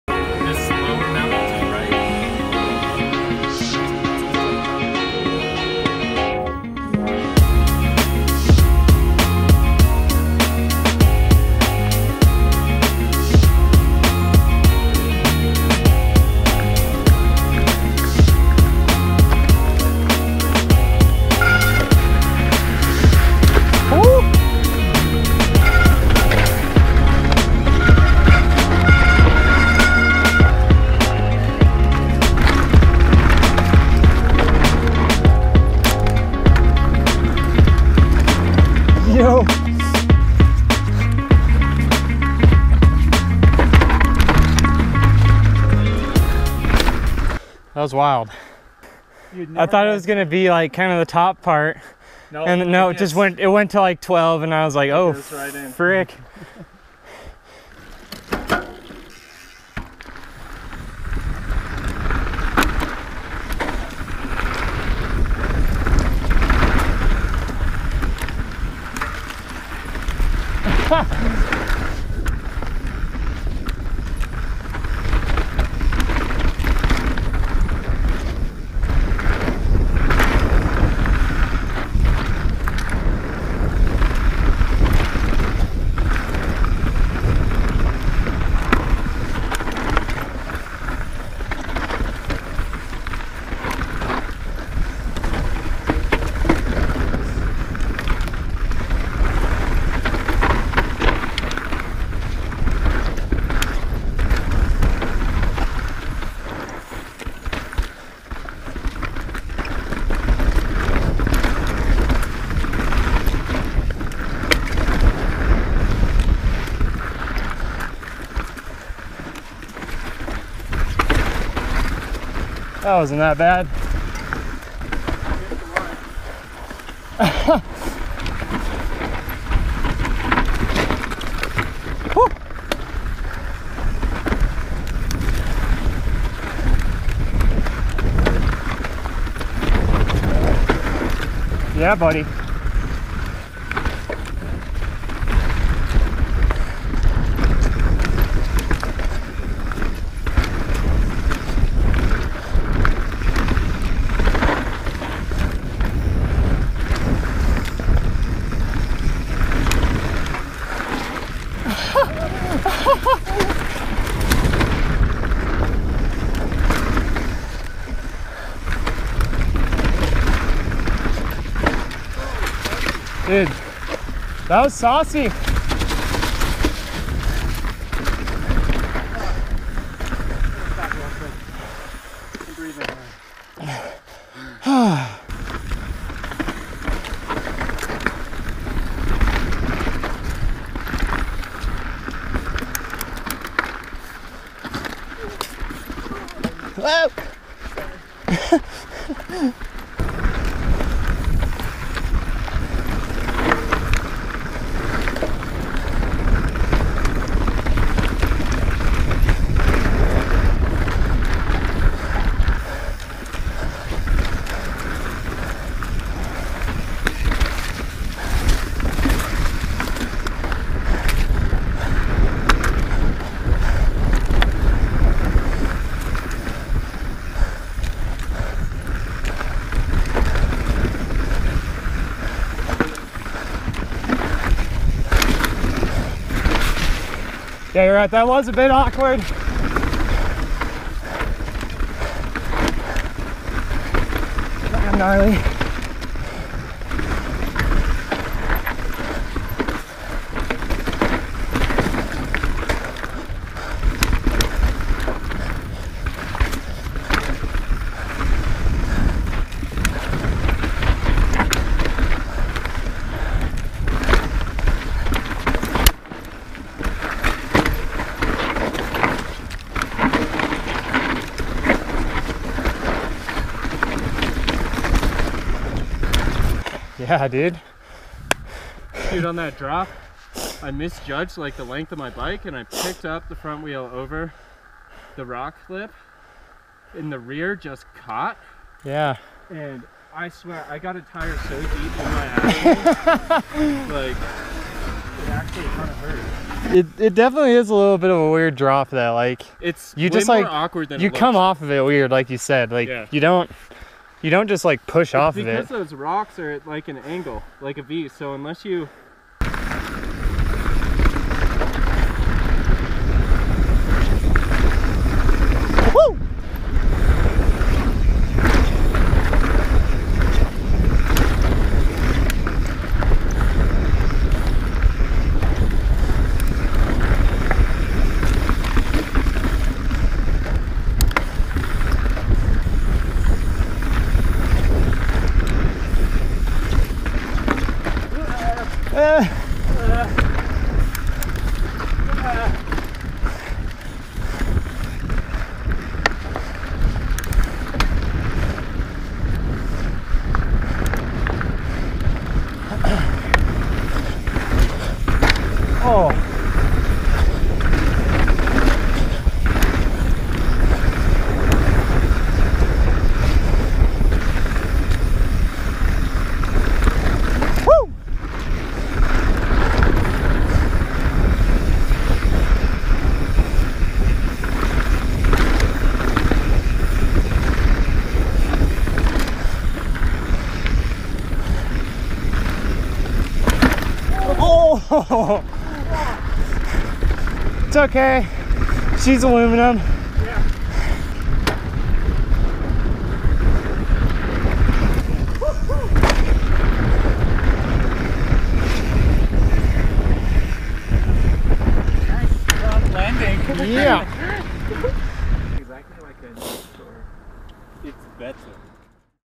Steep and Janky in Flagstaff - Mountain Biking Flagstaff Arizona
Bondo takes us down some classic steep and janky trails in Flagstaff broh.